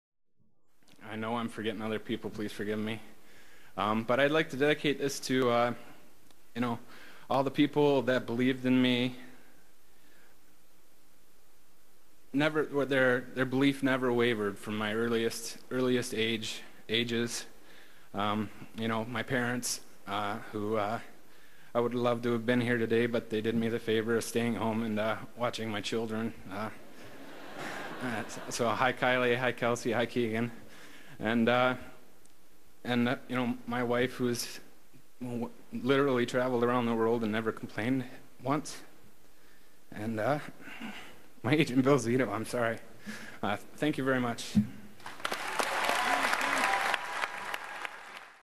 Tim Thomas 2009 Vezina Speech
Acme client Tim Thomas receives one of hockey's top honors, the 2009 Vezina Trophy, and thanks his long time agent Bill Zito.